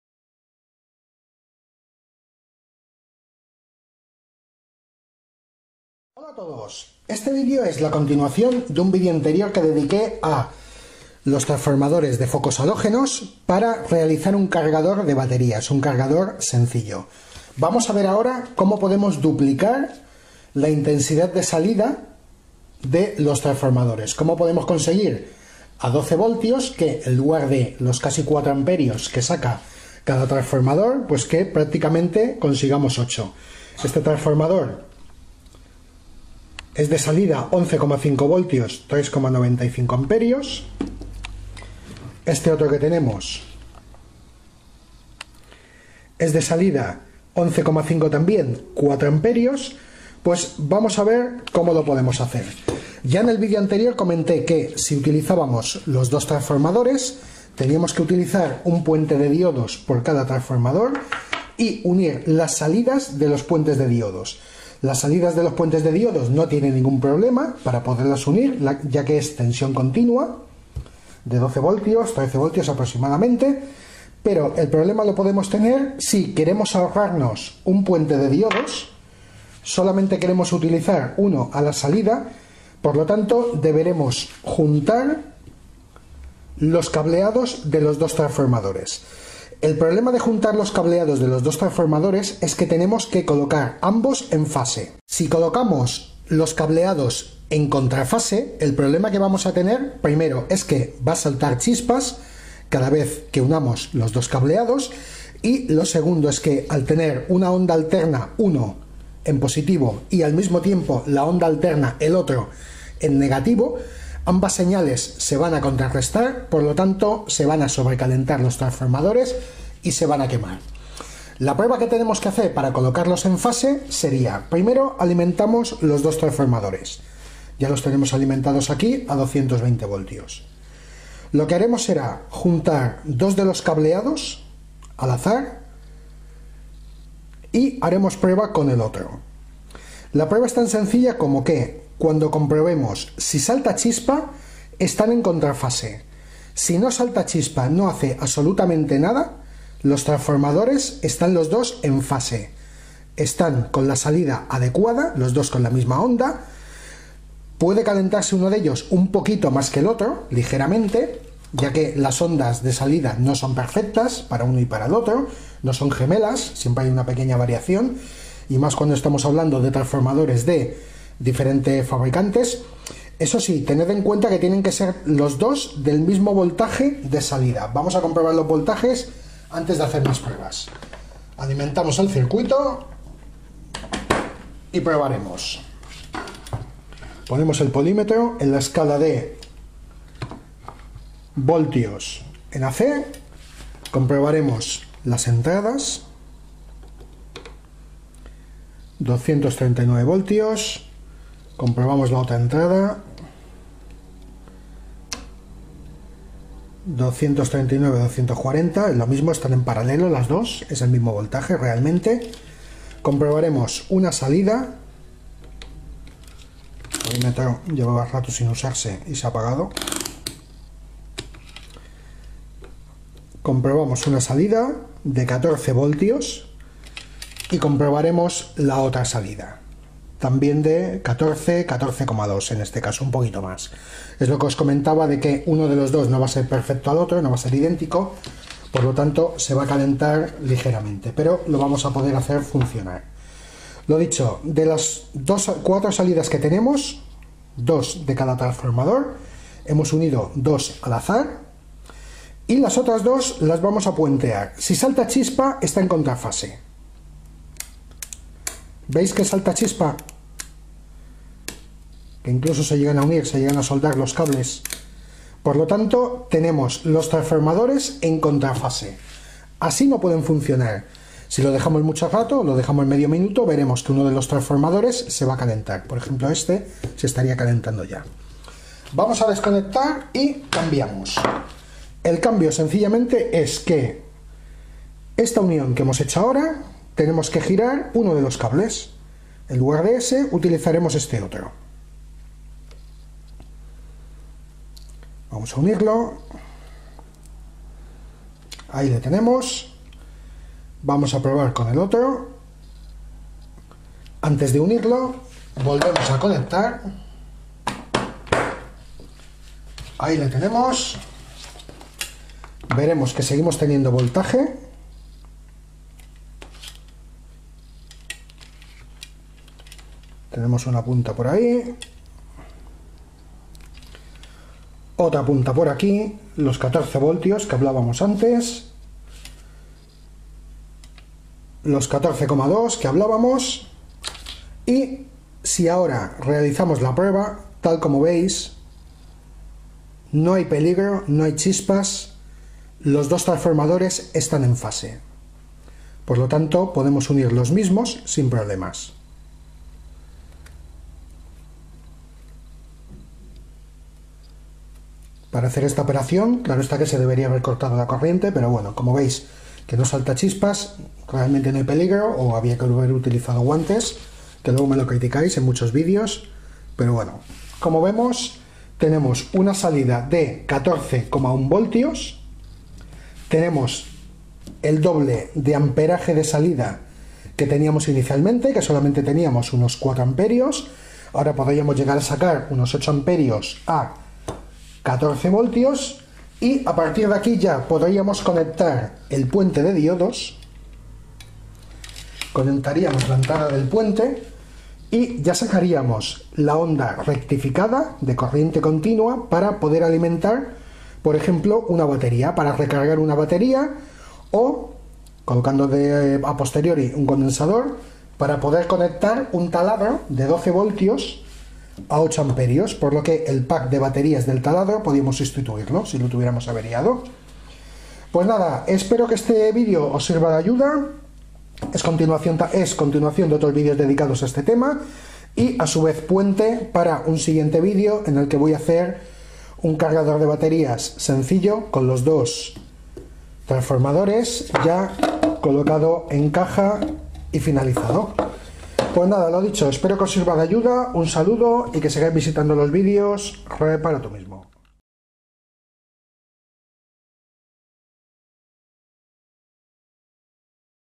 COMO UNIR TRAFOS DE 11,5 V Y DUPLICAR LOS AMPERIOS DE SALIDA CARGADOR BATERIAS
Vídeo explicativo de como conectar 2 transformadores en fase,  para poder duplicar la intensidad de salida de los mismos, usándolos por ejemplo en un cargador de baterías, un vídeo directamente relacionado con 2 series de vídeos que realicé anteriormente.